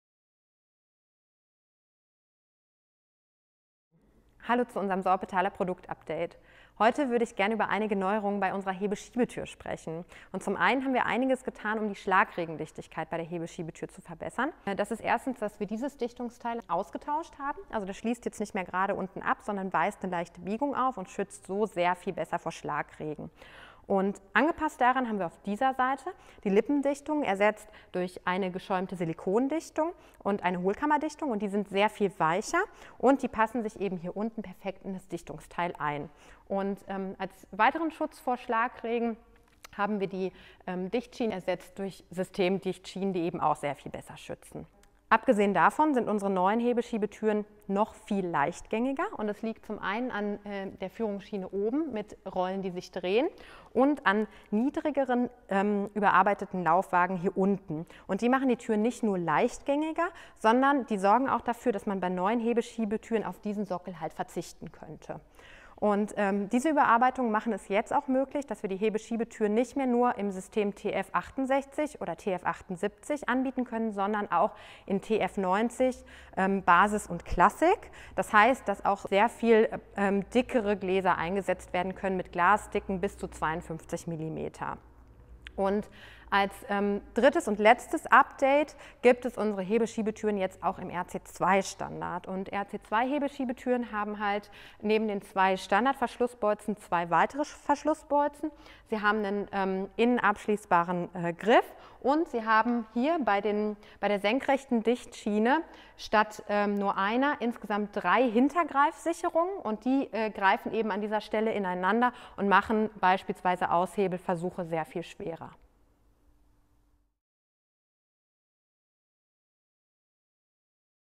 Neue Generation Hebeschiebetür
Wir haben unserer Hebeschiebetür einige Updates verpasst und sie so noch leichtgängiger, noch dichter und noch sicherer gemacht.